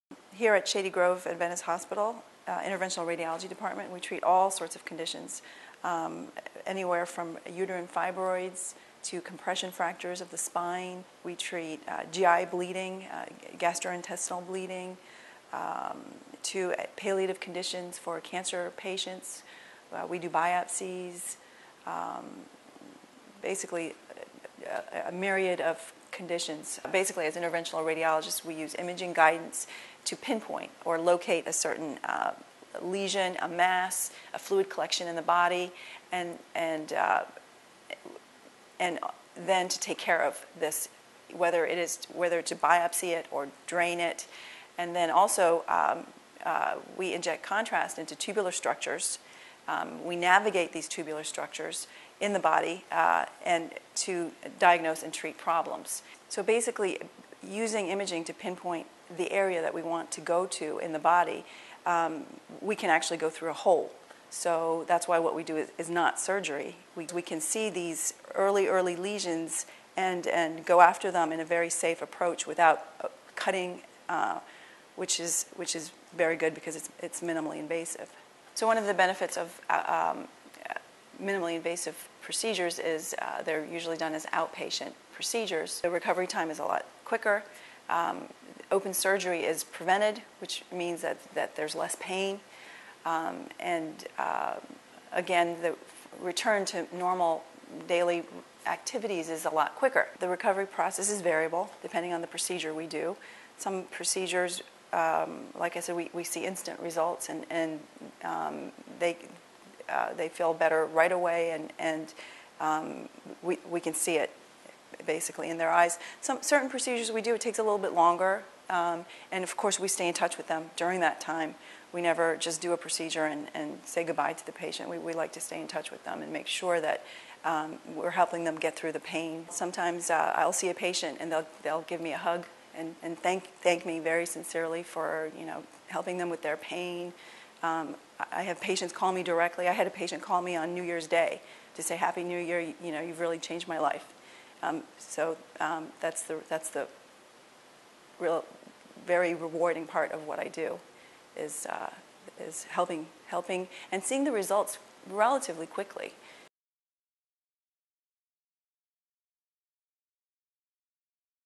Seeing is Relieving: Interventional Radiology Combats Pain Without Surgery
Dr. Zahide Erkmen, interventional radiologist at Shady Grove Adventist Hospital, explains how interventional radiology uses image guidance to combat pain without surgery. Dr. Erkman has performed minimally invasive procedures to help women overcome such things as osteoporosis, reproductive pain or infertility.